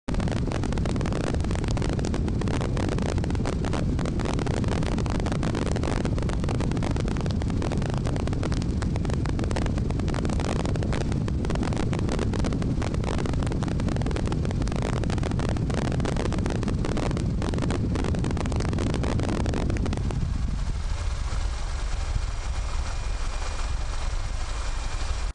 Big Easy
New Orleans bears the devastating effects of Hurricane Katrina.